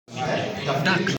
Auf dem Bauernhof
Die Studenten presentieren vor der Klasse